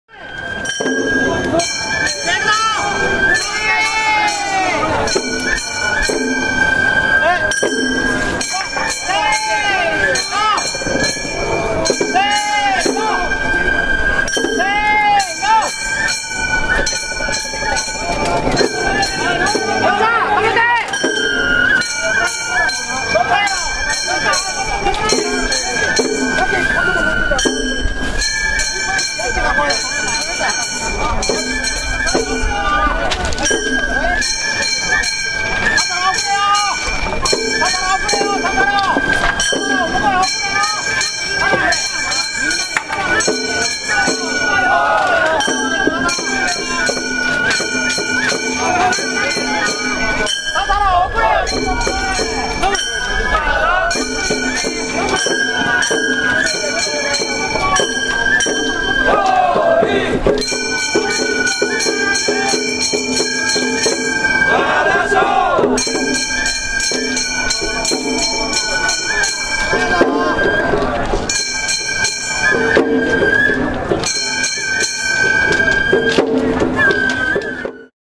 祇園祭の山鉾巡行始まる
京都市で１７日、祇園祭の山鉾巡行が始まった。炎天下の中、豪華な山鉾３２基が都大路を進み、大勢の観光客が見守った。